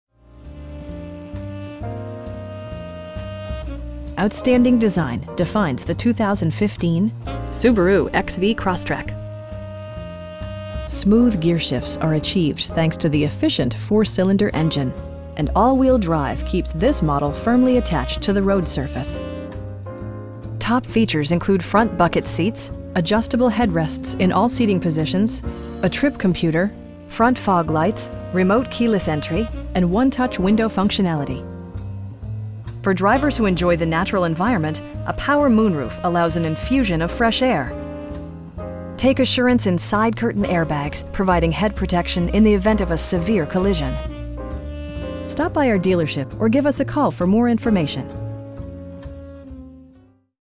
2015 Subaru XV Crosstrek 2.0i Premium w/ Moonroof
Anderson Subaru
7050 Pensacola Blvd.

Outstanding design defines the 2015 Subaru XV Crosstrek. Smooth gearshifts are achieved thanks to the efficient 4 cylinder engine, and all wheel drive keeps this model firmly attached to the road surface. Top features include front bucket seats, adjustable headrests in all seating positions, a trip computer, front fog lights, power door mirrors, power windows, remote keyless entry, and 1-touch window functionality. For drivers who enjoy the natural environment, a power moon roof allows an infusion of fresh air. Take assurance in side curtain airbags, providing head protection in the event of a severe collision. We pride ourselves on providing excellent customer service. Stop by our dealership or give us a call for more information.